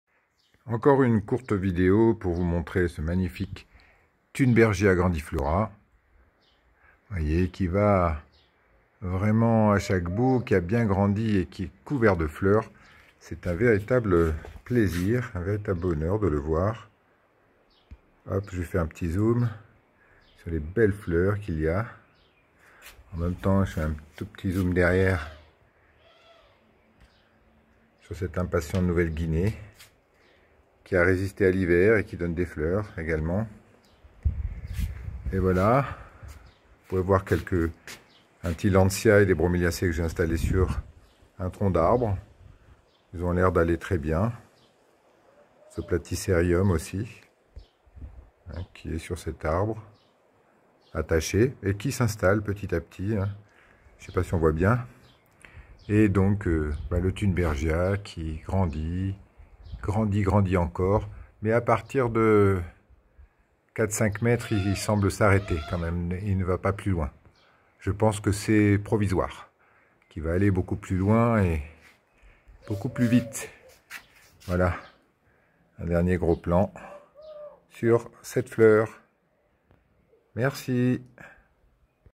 thunbergia grandiflora2
Une seconde video sur le thunbergia grandiflora qui fleurit continuellement et qui semble beaucoup apprécier cet emplacement. Je lui donne de l'engrais assez régulierement ainsi qu'aux plantes qui poussent à ses pieds (gardenia, tibouchina, impatiente de nouvelle guinée)